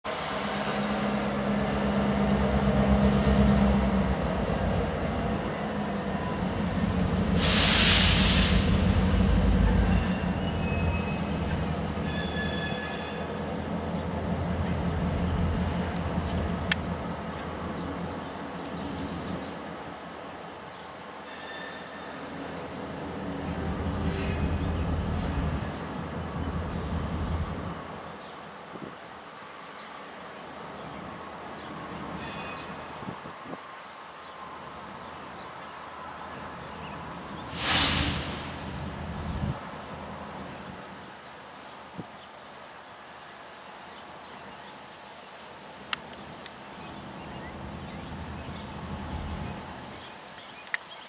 BNSF Geep 2240 & 2837 switch local yard @ 5th Street, Clinton Iowa 6/4/2014 00005
BNSF Geep 2240 & 2837 switch local yard @ 5th Street, Clinton Iowa